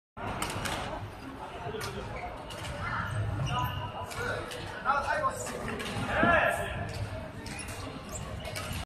Badminton court lighting
It is an honor to announce that we became the solution and ledsportslight provider of LEDlighting renovation for the Shenzhen Bay Sports Center. Tonight, we are disassembling old lamps and installing new LEDhighbaylights for a famous indoor badmintoncourt with 20 courts. Time is very tight as the Government leadership will have a very important game here on weekends. We replaced the 500W MH lamp with 200W LED UFO highbay. Overall illuminance will reach 1000lux with 0.8 uniformity to meet HDTV standards. The specially designed and made anti-glare cover will greatly reduce the glare value and bring you extremely comfortable light. Next month we will carry out lighting renovations on 25 outdoor basketball courts. More photos and videos will be shared instantly.

The big badminton center in Shenzhen Bay Sports Complex we have installed our ledhighbaylight last weekend. Over 200pcs 200W LED replaced 500W MH highbay lamps to achieve average 500lux. Ultra-low GR value and outstanding light quality make clients and players quite happy.